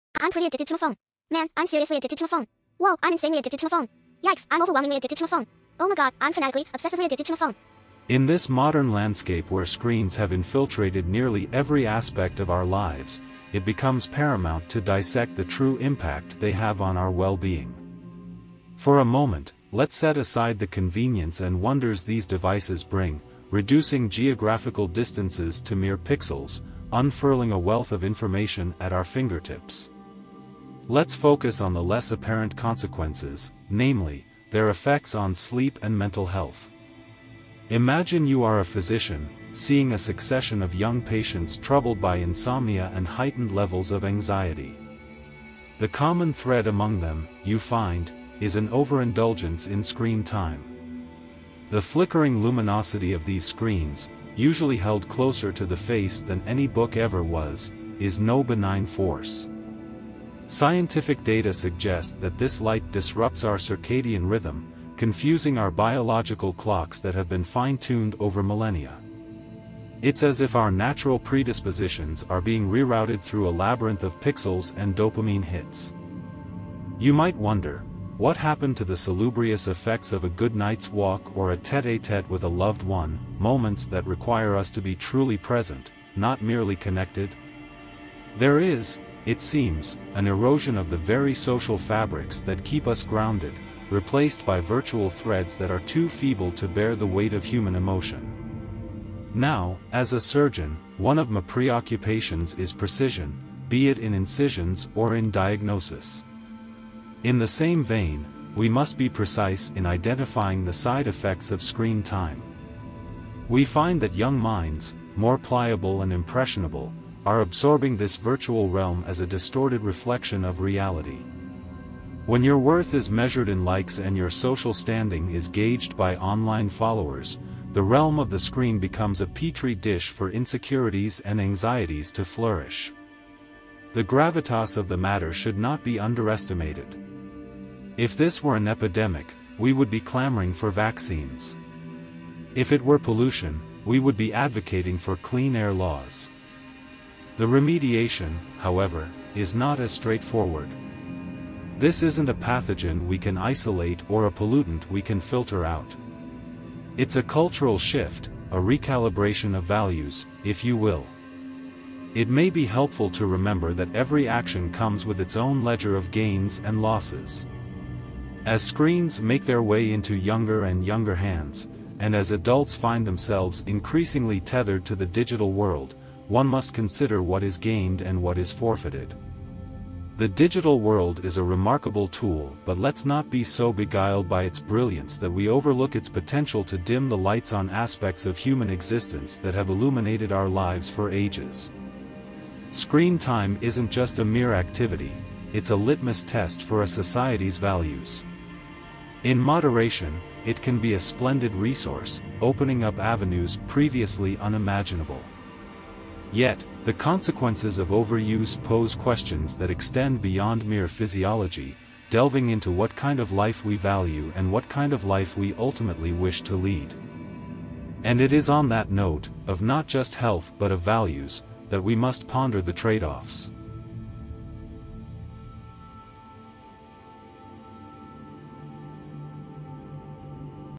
The Real Reason You Can't Put Your Phone Down — It's Not What You Think!
Greetings, esteemed viewers. We find ourselves living in an epoch where technological wonders appear to have eclipsed the human spirit. The device you hold in your hand—so-called 'smartphones'—may be a modern marvel, but have you ever considered the weight it exerts on your life?

In an era that once championed the rugged individualism portrayed by Ronald Reagan, we find ourselves increasingly dependent on these electronic leashes. They promise efficiency and connectivity but deliver—at best—superficial relationships and a squandering of the most non-renewable resource: time.

We once aspired to be like the self-made entrepreneurs of Wall Street, or to follow in the footsteps of statesmen who spent lifetimes honing the art of governance and public service. Today, the specter of phone addiction raises questions about what we're truly accomplishing with our lives.

But before you jump to conclusions, allow this presentation to elucidate a point of view you may not have considered. You see, the reason you can't put down your phone isn't solely due to the dopamine hits from 'likes' or 'follows.' No, the issue digs deeper into the very fabric of human psychology and the free market forces that exploit it. The purveyors of these devices and apps are not just selling convenience; they are auctioning off your attention, the most precious commodity in a capitalistic society.

Join me in this cerebral exploration as we navigate the labyrinthine issues surrounding phone addiction, from the invisible hand that perpetuates consumer behavior to the breakdown of genuine human connections, which were once the bedrock of a well-functioning society.

So if you find yourself scrolling mindlessly through a feed when you could be reading Milton Friedman or engaging in community service, I urge you to take a pause. This video aims to awaken you to the real reasons behind your compulsive phone use, and perhaps, inspire you to reclaim the purposeful life you are fully capable of leading.